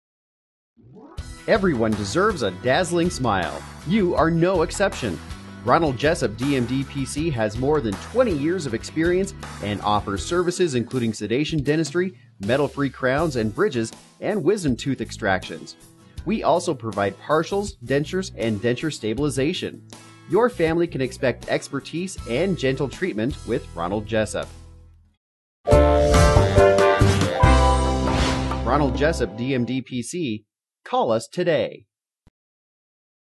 Ronald Jessup, DMD, PC
Everyone deserves a dazzling smile.  You are no exception. Ronald Jessup, DMD, PC has more than 20 years of experience, and offers services including sedation dentistry, metal-free crowns and bridges, and wisdom tooth extractions.  We also provide partials, dentures, and denture stabilization. Your family can expect expertise and gentle treatment with Ronald Jessup.